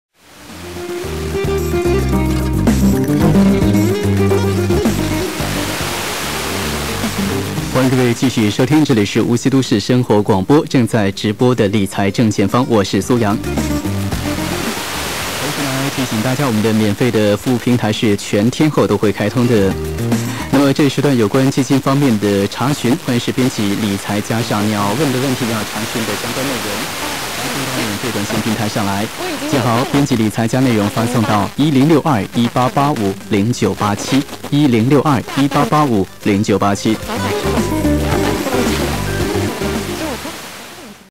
無錫都市生活広播 98.7MHz 2008年06月 Eスポ受信
AR5000＋CLP-5130-1

無錫都市生活広播　98.7MHz
无锡都市生活广播
中国　江蘇省無錫市
June　2008

98.7MHzは中央人民広播電台経済之声に転用され、都市生活広播は88.1MHzへQSYし、
2022年から無錫市錫山区融媒体中心綜合広播のIDが出るようになった